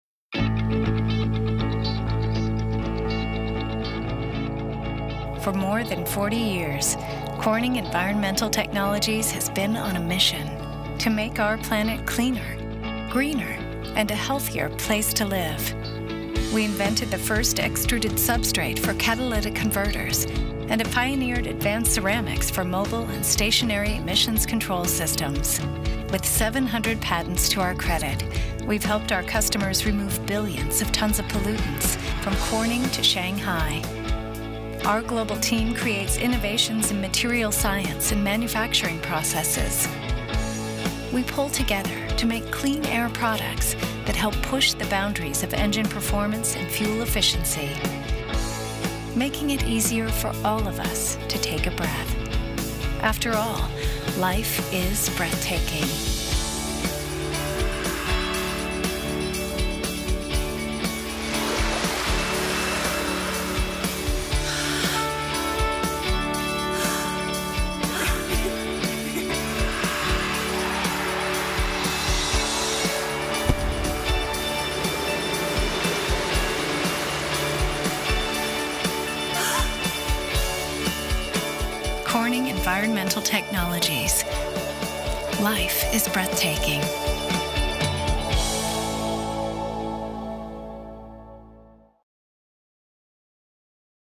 Life is Breathtaking
In the exhaust systems of cars and trucks, Corning’s products are hard at work to help make our world cleaner, greener, and a healthier place to live. Our ceramic substrates and filters are complex, but their purpose is clear. It all comes down to making the air we breathe cleaner, so we can all enjoy life’s breathtaking moments. More